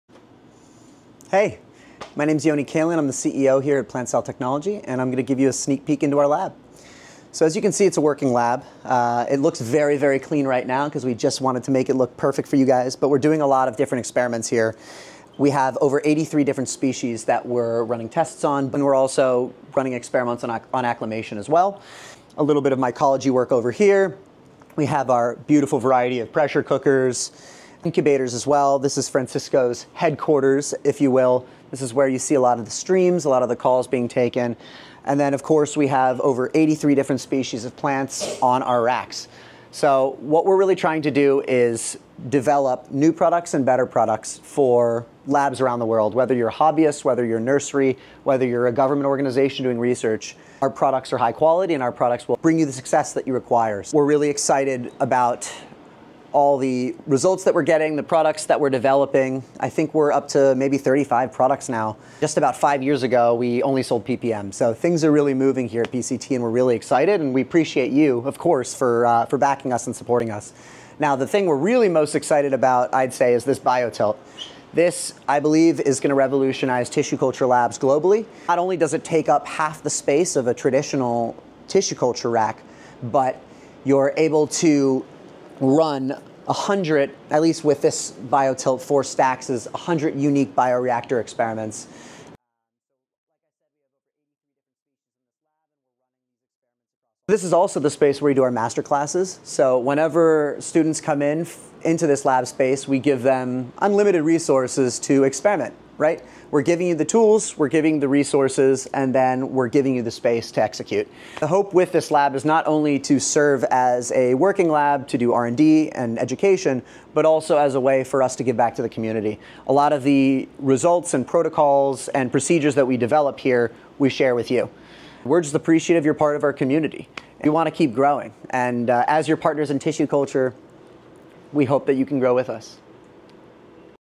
BTS from our DC Lab with our CEO! 🥳🧑‍🔬🥼
Our DC Lab Tour with our CEO, Yoni Kalin! 🧑‍🔬

Join our CEO, as he takes us on an exclusive tour of our bustling lab in Washington DC.

Here’s what’s happening behind the scenes:

🌿 Dynamic Research Environment: Our lab is a hub of activity with over 83 different species under examination. From advanced tissue culture experiments to mycology studies, we're pushing the boundaries of plant science.

🧪 Innovation in Action: Yoni showcases the heart of our operations—Francisco's headquarters. This is where many of our live streams and crucial discussions take place, surrounded by essential lab equipment like incubators and pressure cookers.

🌱 Education and Development: The lab also serves as a classroom for our master classes, providing students with unlimited resources to explore and innovate.

💡 Introducing the BioTilt™: Yoni highlights our latest breakthrough, the BioTilt™, a game-changer for tissue culture labs worldwide. This compact yet powerful system can manage up to 100 bioreactor experiments, optimizing space and enhancing productivity.

🌐 Growth and Community: From starting with a single product, PPM™, to now offering 35 innovative solutions, PCT has grown immensely thanks to your support. Our goal is to continue developing high-quality products that meet the diverse needs of our global community, from hobbyists to government research labs.

📈 Giving Back: Everything we learn and develop in our lab is shared with you through our newsletters, SOPs, and reports, contributing to a richer, more collaborative tissue culture community.

We're grateful for your support and excited to keep growing with you as your dedicated partners in tissue culture.